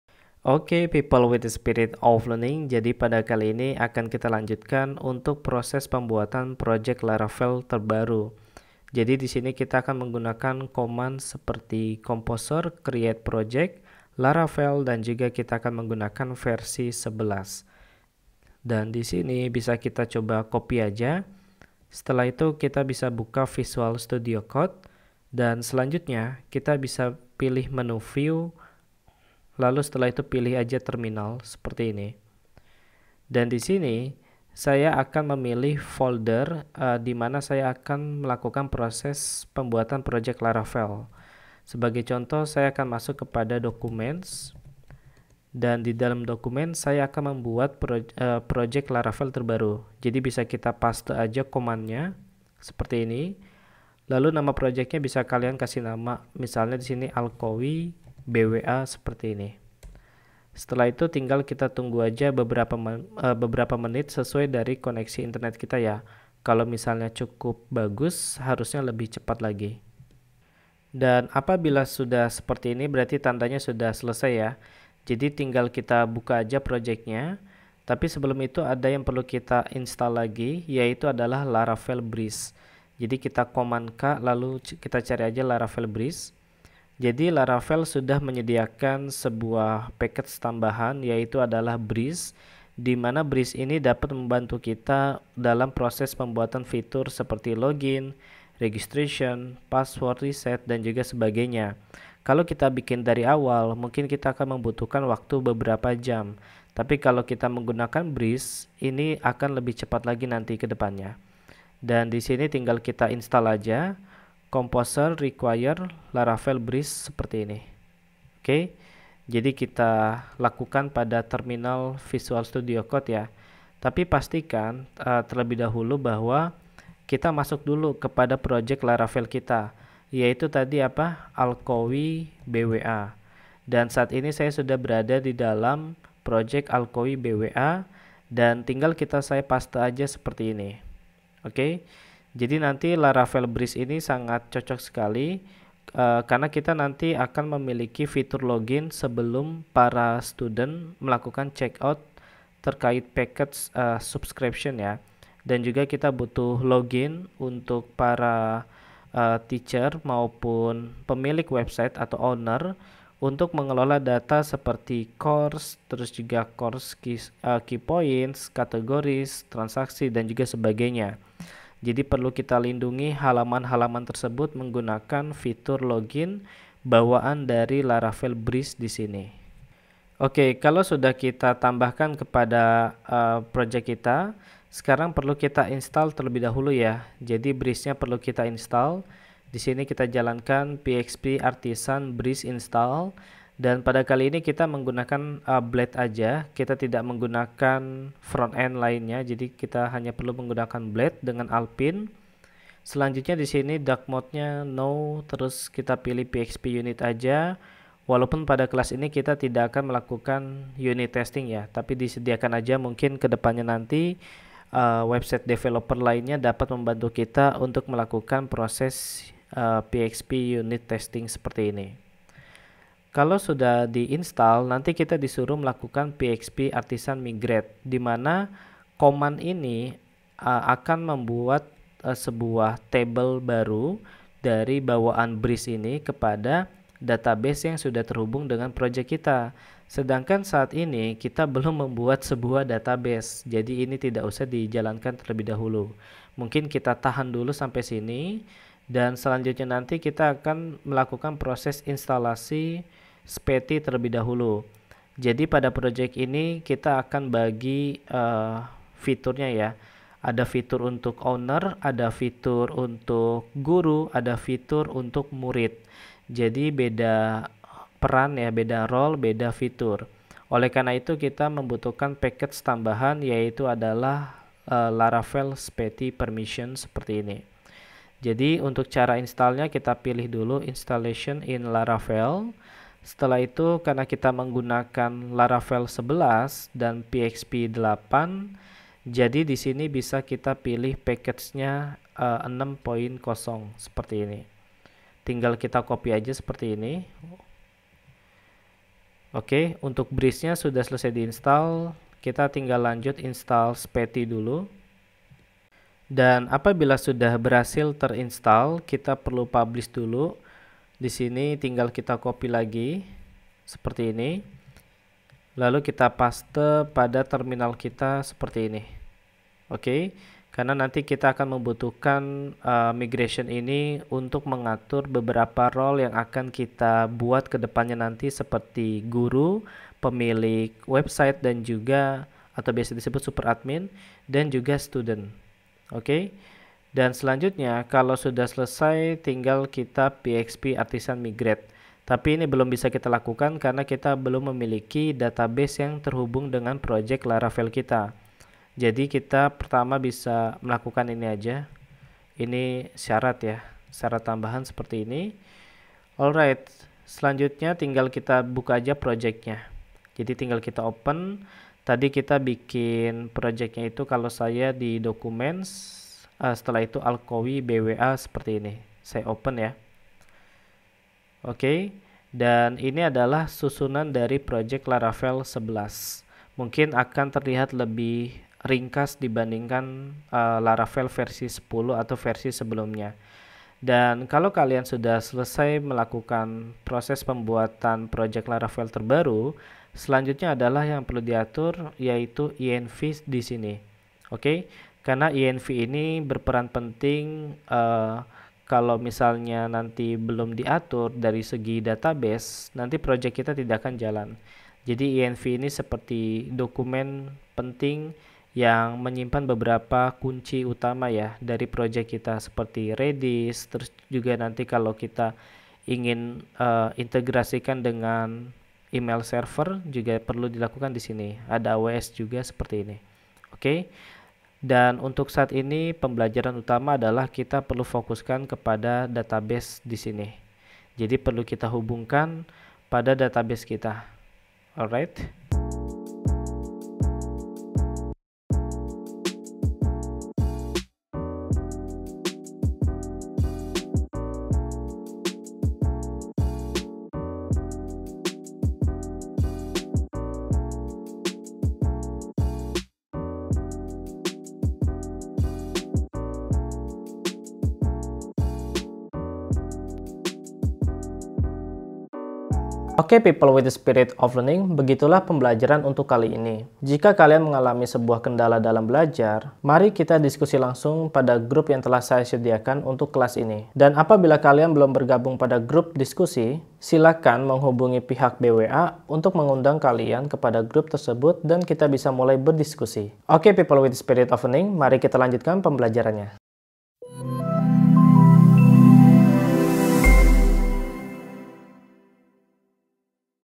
Belajar PHP Laravel 11 Bikin Web E-Learning dan Subscription Part 4
Hi people with the spirit of learning, pada kali ini kita akan mempelajari bagaimana caranya install laravel breeze dan spati permission untuk mengelola beberapa roles pada setiap user di dalam projek website e-learning menggunakan framework php laravel.

pelajari materi selengkapnya pada Kelas Online
Laravel 11, Breeze, Spatie: Bikin Web Platform Online Course: